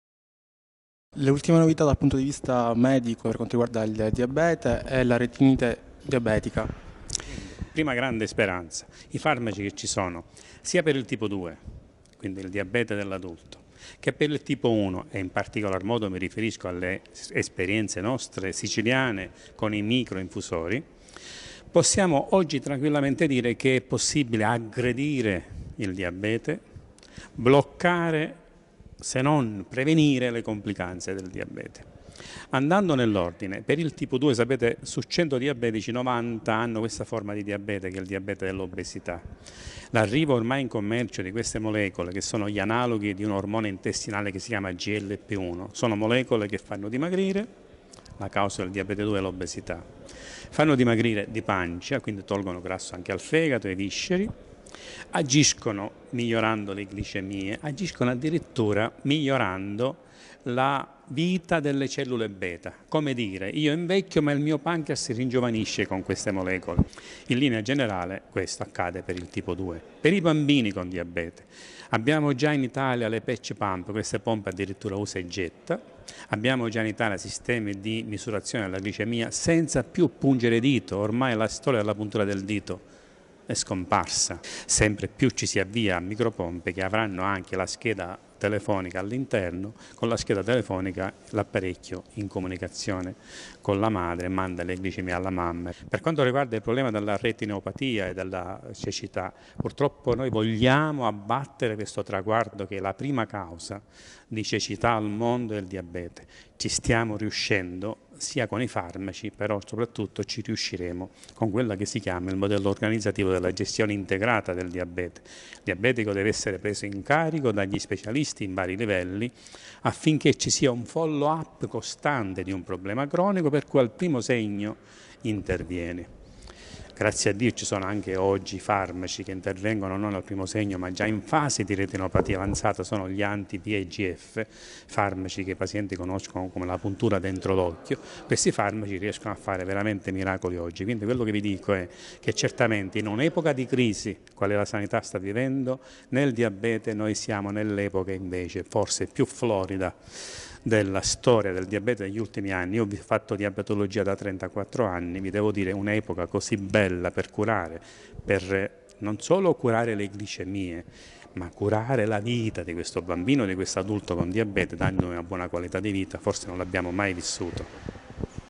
Dr. Vincenzo Provenzano - Diabetologo
Bloccare il diabete e prevenirlo si può, così come riferisce il Dottore Vincenzo Provenzano, diabetologo responsabile dell’UoC malattie metaboliche e del ricambio dell’Ospedale civico di Partitico. Con l’arrivo delle nuove molecole si riesce a fermare il percorso progressivo del diabete migliorando la qualità della vita ed evitando gravi conseguenze non solo per quanto riguarda la retinopatia ma anche più in generale a prevenire conseguenze più gravi per l’organismo umano.

Servizio di Daniele Inglese